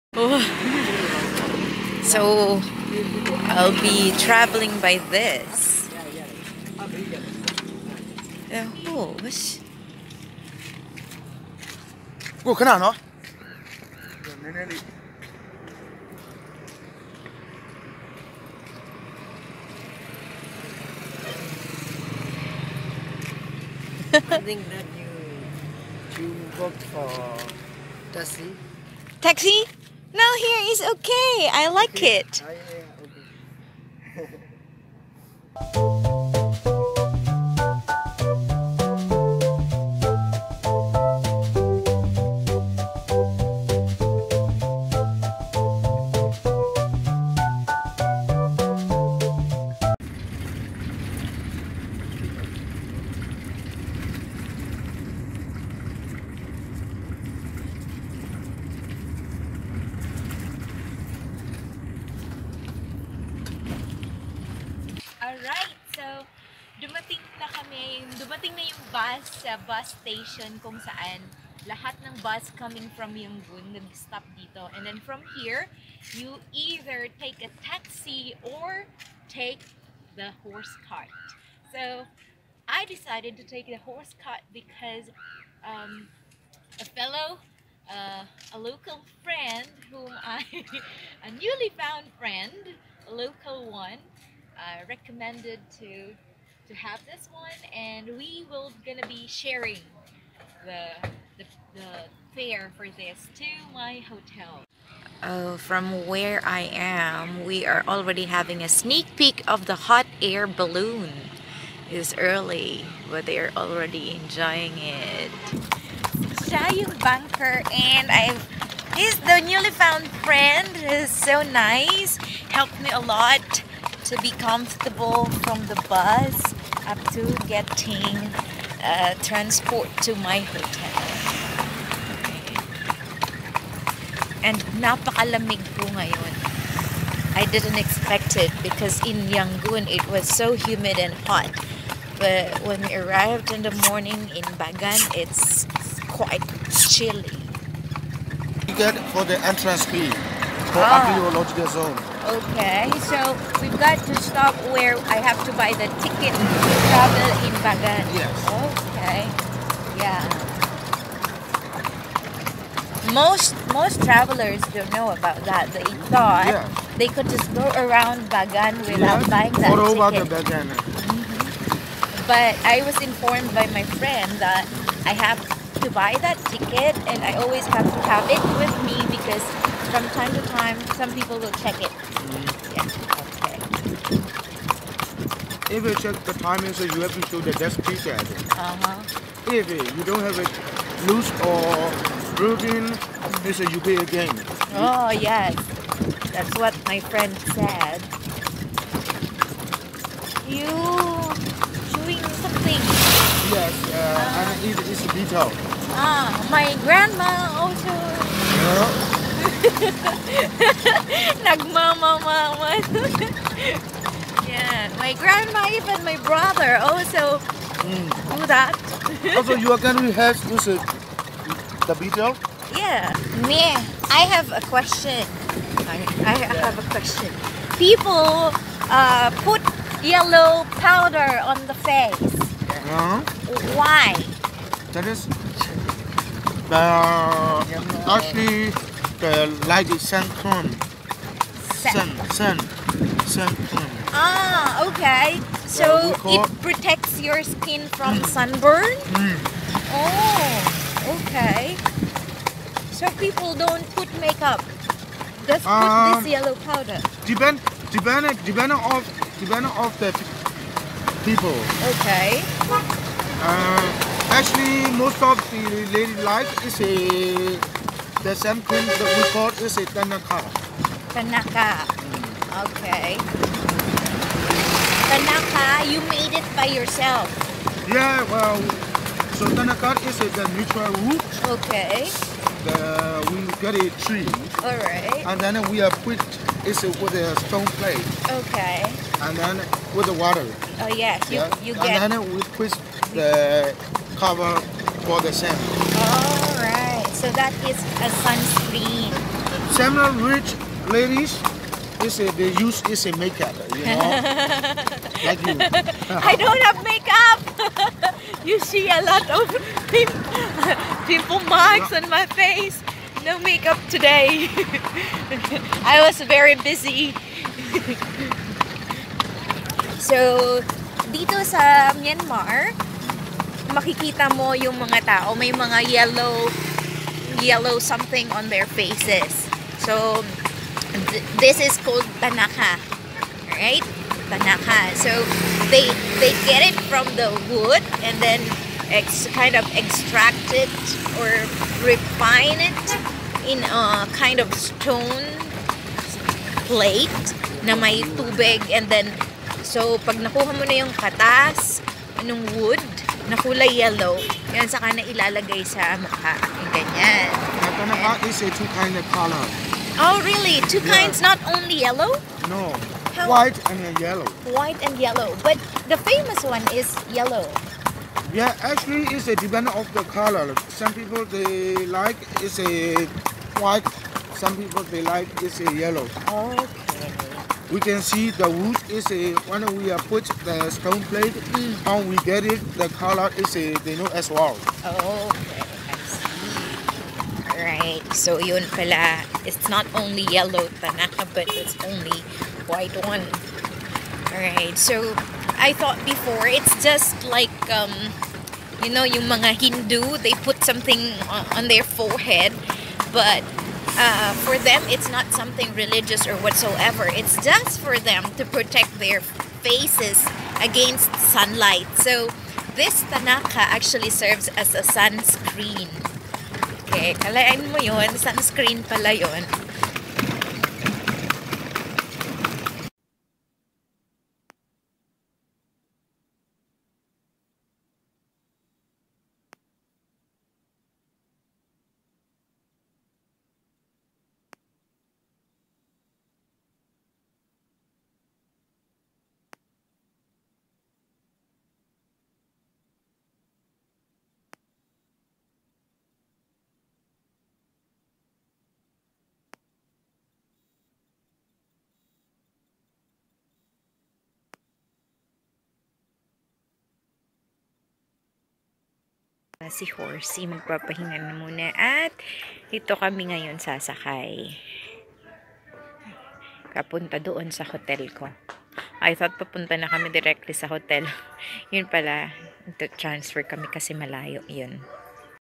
Horse Cart Ride in Bagan (I didn't know it until I was brought to it)
While on the way to my hotel in Bagan, I got to know more insights about the local's life through the English Speaking driver and the newly found local friend which strengthen my Idea how kind Myanmar people are.